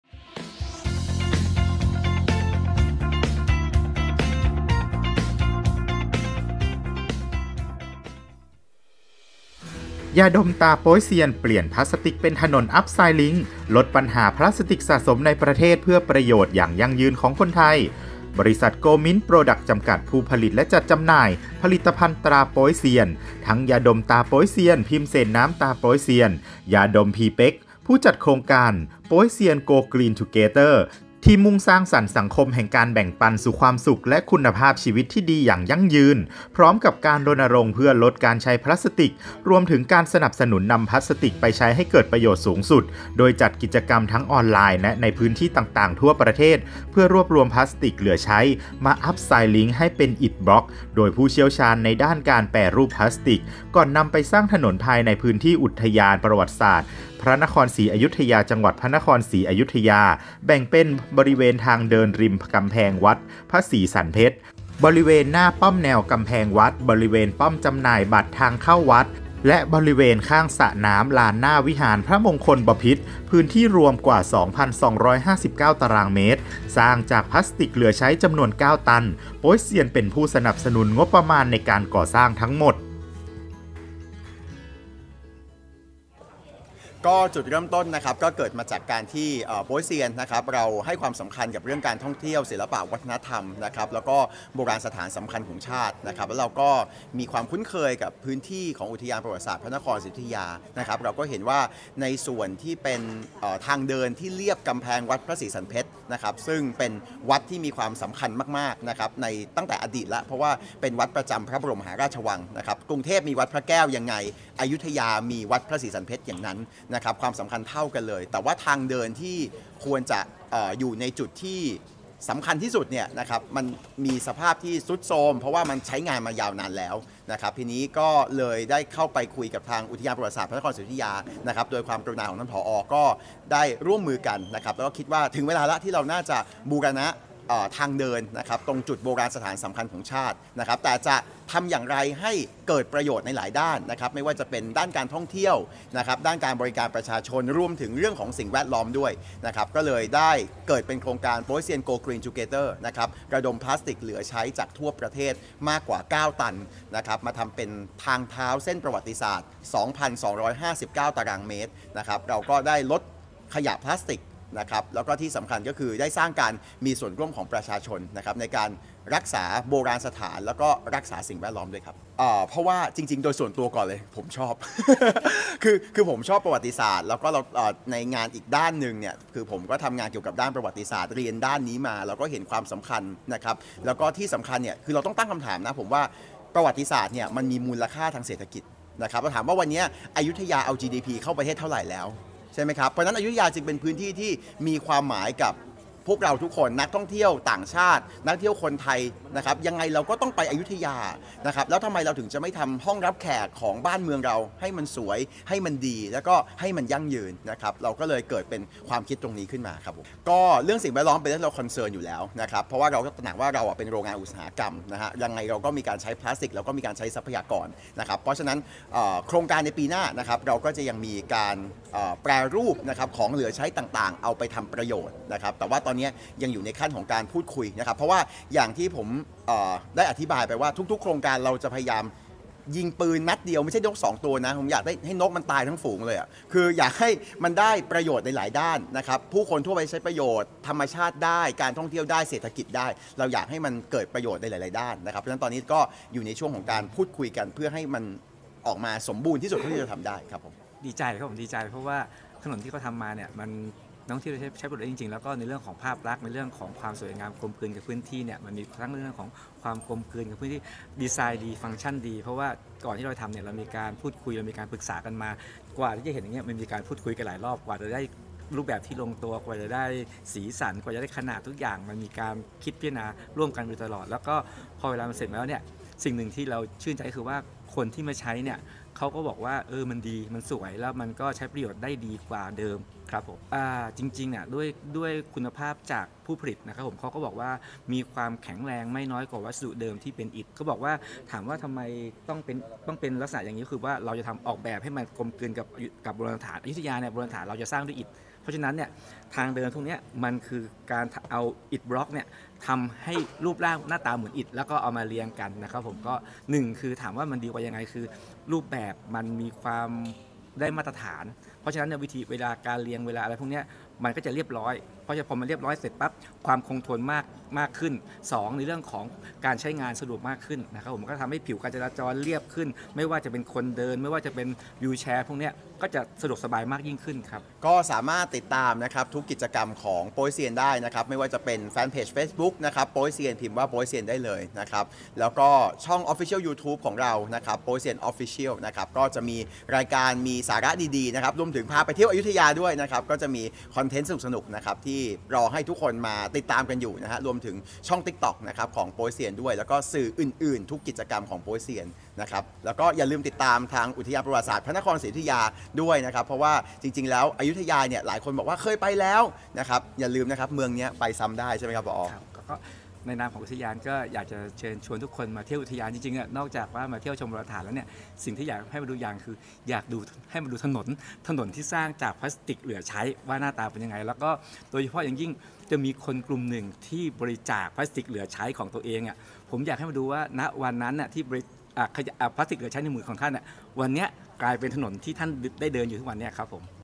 ยาดมตราโป๊ยเซียน เปลี่ยนพลาสติกเป็นถนน UPCYCLING ลดปัญหาพลาสติกสะสมในประเทศ
ยาดมตราโป๊ยเซียน เปลี่ยนพลาสติกเป็นถนน UPCYCLING ลดปัญหาพลาสติกสะสมในประเทศ เพื่อประโยชน์อย่างยั่งยืนของคนไทยา

บริษัท โกลด์ มิ้นท์ โปรดักส์ จำกัด ผู้ผลิตและจัดจำหน่าย ผลิตภัณฑ์ตราโป๊ยเซียน ทั้งยาดมตราโป๊ยเซียน พิมเสนน้ำตราโป๊ยเซียน ยาดมพีเป๊กซ์ ผู้จัดโครงการ “Poysian Go Green Together”
ที่มุ่งสร้างสรรค์สังคมแห่งการแบ่งปันสู่ความสุขและคุณภาพชีวิตที่ดีอย่างยั่งยืน
พร้อมกับการรณรงค์เพื่อลดการใช้พลาสติก รวมถึงสนับสนุนการนำพลาสติกไปใช้ให้เกิดประโยชน์สูงสุด
โดยจัดกิจกรรมทั้งออนไลน์และในพื้นที่ต่างๆ ทั่วประเทศ เพื่อรวบรวมพลาสติกเหลือใช้มา UPCYCLING
ให้เป็นอิฐบล็อกโดยผู้เชี่ยวชาญในด้านการแปรรูปพลาสติก
ก่อนนำไปสร้างถนนภายในพื้นที่อุทยานประวัติศาสตร์พระนครศรีอยุธยา จังหวัดพระนครศรีอยุธยา
แบ่งเป็น บริเวณทางเดินริมกำแพงวัดพระศรีสรรเพชญ์ บริเวณหน้าป้อมแนวกำแพงวัด
บริเวณป้อมจำหน่ายบัตรทางเข้าวัด และบริเวณข้างสระน้ำลานหน้าวิหารพระมงคลบพิตร
พื้นที่รวมกว่า 2,259 ตารางเมตร สร้างจากพลาสติกเหลือใช้จำนวน 9 ตัน
โป๊ยเซียนเป็นผู้สนับสนุนงบประมาณในการก่อสร้างทั้งหมด

ล่าสุด ดร.ณัฐพงศ์ ลาภบุญทรัพย์ กรรมการและที่ปรึกษาบริษัท โกลด์ มิ้นท์ โปรดักส์ จำกัด จัดงานส่งมอบถนน โครงการ “Poysian Go Green Together”อย่างเป็นทางการ ซึ่งได้รับเกียรติจาก นายภัทรพงษ์ เก่าเงิน ผู้อำนวยการอุทยานประวัติศาสตร์พระนครศรีอยุธยา กรมศิลปากร กระทรวงวัฒนธรรม เป็นผู้รับมอบ พร้อมด้วย คุณหัทยา สารลิขิต ผู้จัดการฝ่ายโฆษณา บริษัท โกลด์ มิ้นท์ โปรดักส์ จำกัด ผศ.ดร. เวชสวรรค์ หล้ากาศ เจ้าของโครงการบล็อกปูถนนรีไซเคิลจากมหาวิทยาลัยราชภัฏเชียงใหม่ และคณะเจ้าหน้าที่อุทยานประวัติศาสตร์พระนครศรีอยุธยา มาร่วมงาน ณ เวิร์คพอยท์ สตูดิโอ จ.ปทุมธานี

โดยถนนเส้นประวัติศาสตร์ Poysian Upcycling พื้นที่ในอุทยานฯ แบ่งเป็น บริเวณทางเดินริมกำแพงวัดพระศรีสรรเพชญ์ บริเวณหน้าป้อมแนวกำแพงวัด บริเวณป้อมจำหน่ายบัตรทางเข้าวัด และบริเวณข้างสระน้ำลานหน้าวิหารพระมงคลบพิตร พื้นที่รวมกว่า 2,259 ตารางเมตร สร้างจากพลาสติกเหลือใช้จำนวน 9 ตัน ซึ่งโป๊ยเซียนเป็นผู้สนับสนุนงบประมาณในการก่อสร้างทั้งหมด เพื่อมอบเป็นสาธารณประโยชน์แก่ชุมชนและนักท่องเที่ยวทั้งชาวไทยและต่างชาติ ที่เดินทางมาท่องเที่ยว ณ อุทยานประวัติศาสตร์พระนครศรีอยุธยา

ดร.ณัฐพงศ์ ลาภบุญทรัพย์ กรรมการและที่ปรึกษาบริษัทฯ เผยว่า “เสร็จสมบูรณ์เป็นที่เรียบร้อยแล้วครับกับความภูมิใจของพวกเราครอบครัวโป๊ยเซียนและพี่น้องคนไทยทุกคน ที่ส่งพลาสติกเหลือใช้มาร่วมสร้างถนนเส้นประวัติศาสตร์ครั้งนี้ด้วยกัน ในนามของผลิตภัณฑ์ตราโป๊ยเซียน นอกเหนือจากการพัฒนาผลิตภัณฑ์เพื่อให้ตอบโจทย์ผู้บริโภคมากที่สุด การได้เป็นส่วนหนึ่งสร้างสังคมให้น่าอยู่ก็เป็นทิศทางการดำเนินงานของเรา ซึ่งตลอดปี 2567 นี้ เราได้ลงพื้นที่ ลงมือทำกิจกรรมต่างๆ เพื่อลดปัญหาขยะพลาสติกที่เกิดขึ้นในประเทศไทย ถนนเส้นประวัติศาสตร์ Poysian Upcycling สามารถช่วยลดปัญหาพลาสติกสะสมในประเทศได้มากถึง 9 ตัน และยังเป็นการนำพลาสติกมาใช้ให้เกิดประโยชน์สูงสุดอีกด้วย ผมหวังเป็นอย่างยิ่งว่า ถนนเส้นนี้จะก่อให้เกิดประโยชน์สูงสุดให้แก่คนไทยทั้งประเทศ และขอบคุณทุกๆ ท่าน ที่สนับสนุนโป๊ยเซียนด้วยดีเสมอมา ไม่ว่าโป๊ยเซียนจะจัดกิจกรรมอะไร ลงพื้นที่ไปจังหวัดไหน หรือแม้แต่ในออนไลน์ ก็ได้รับการตอบรับจากพี่น้องชาวไทยด้วยดีเสมอมา ผมจึงอยากมอบความสุขและความภาคภูมิใจในครั้งนี้ให้กับพี่น้องชาวไทยทุกคนครับ”

ติดตามกิจกรรม “Poysian Go Green Together” และ กิจกรรมต่างๆ ของผลิตภัณฑ์ตราโป๊ยเซียน ได้ที่ Facebook Fanpage : โป๊ยเซียน-POYSIAN หรือ Line OA @Poysian